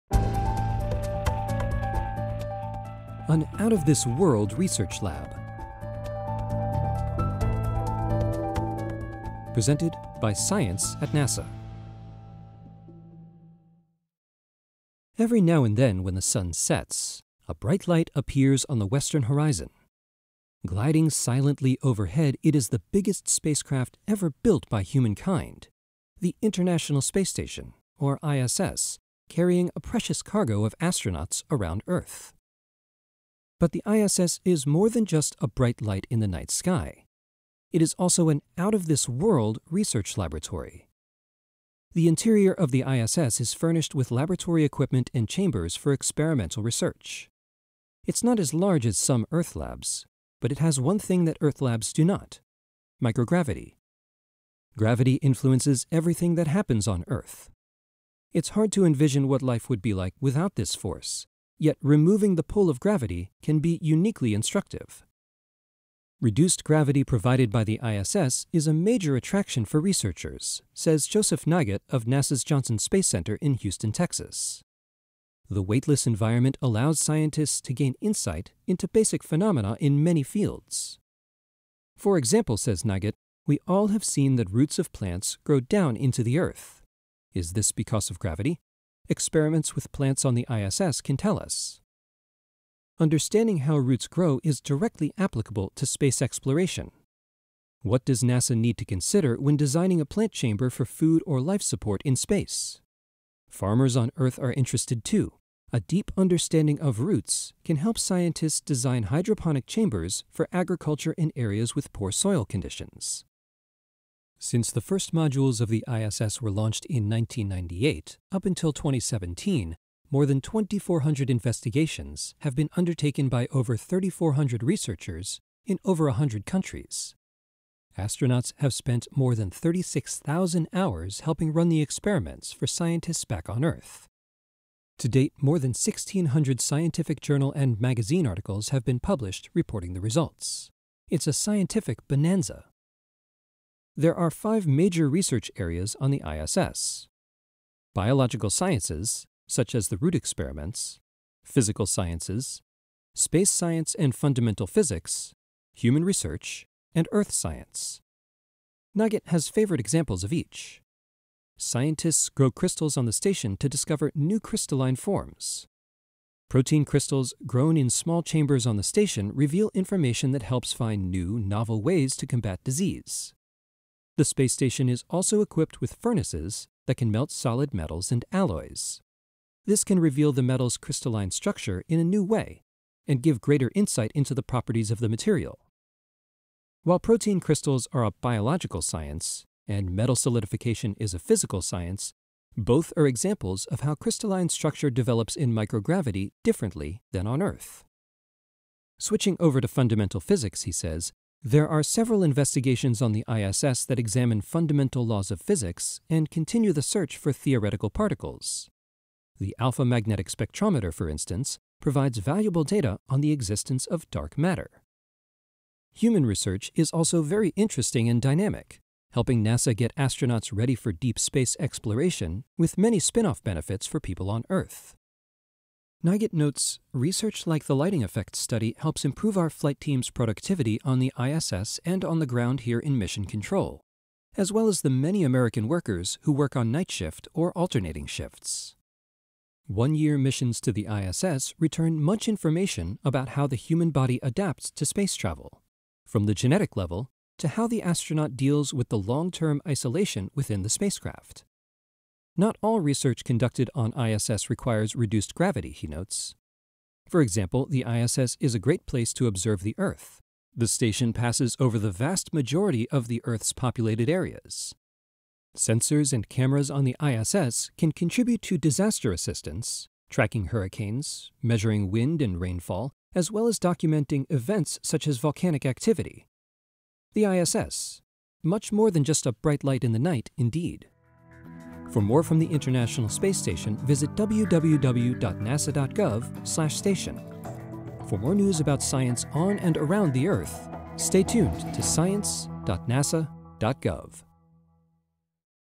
ScienceCasts: An Out of This World Research Lab
The International Space Station is more than just a bright light in the night sky. It is also an out-of-this-world research laboratory.